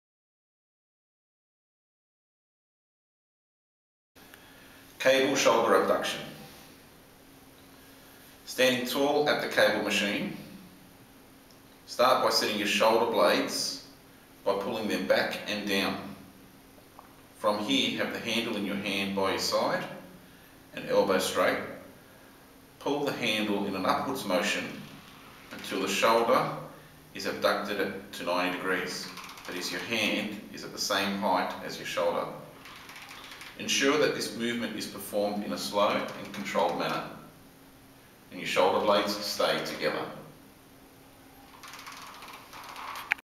Cable shoulder abduction
Setting the scapular while working the abductors